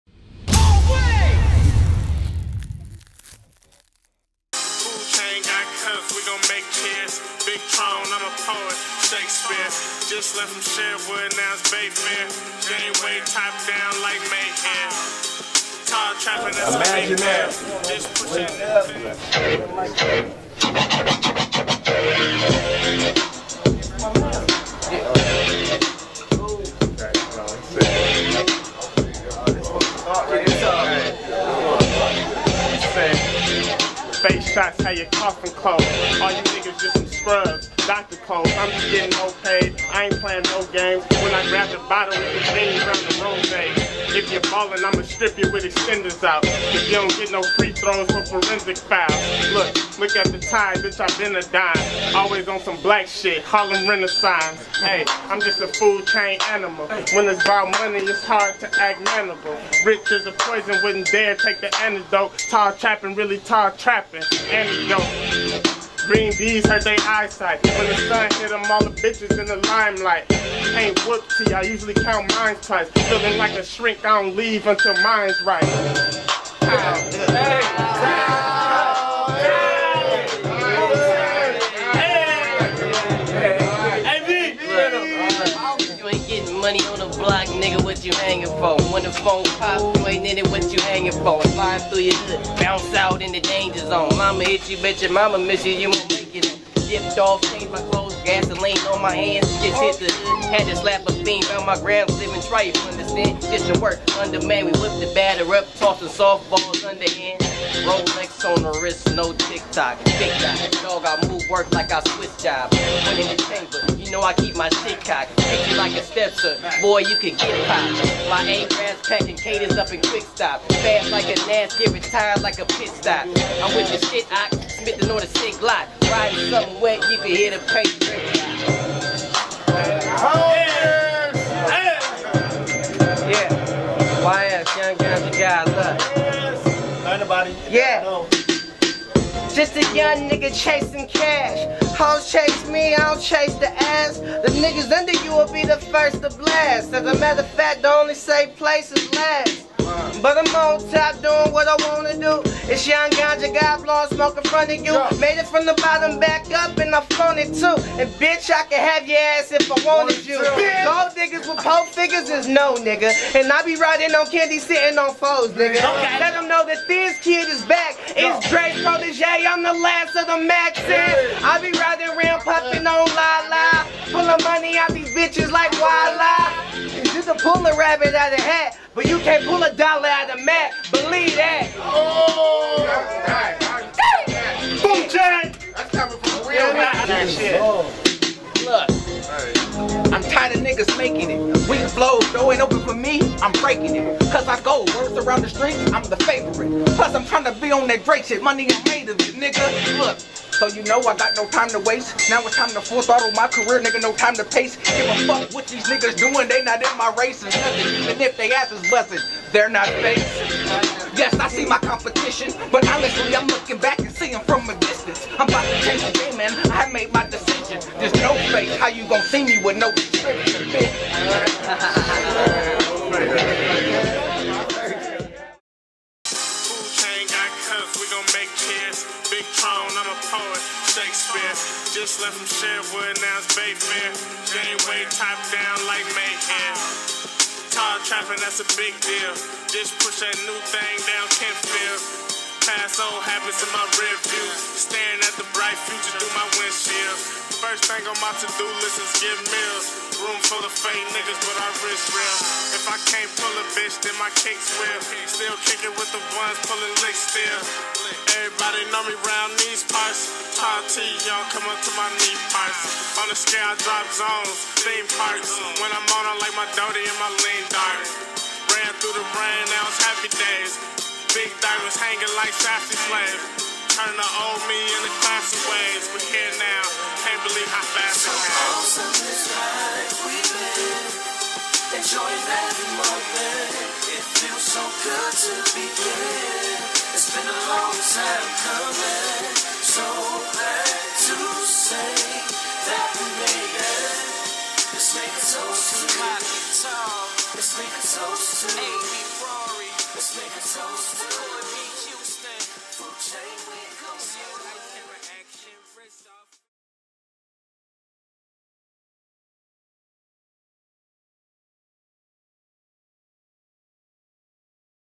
209 Hip Hop Cypher Pt 1
Tall T, AB Hogish, YS Gonja & 2Face. 209 Hip Hop Cypher pt 1